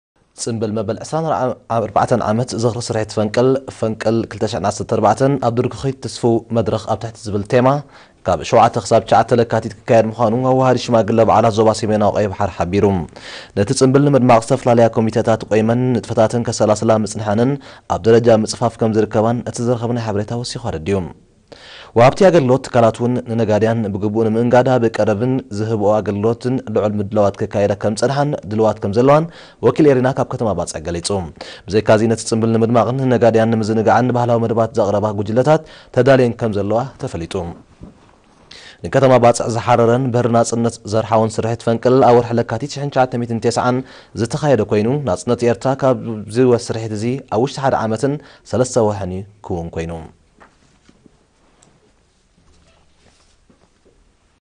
Eri-TV - Commemoration of Operation Fenkil to be conducted from 7 to 9 February
- Eritrean News: The Holidays Coordinating Committee in the Northern Red Sea region pointed out that Operation Fenkil would be commemorated from 7 to 9 February under the theme "FENKIL 2014: On Threshold of a Promising Phase."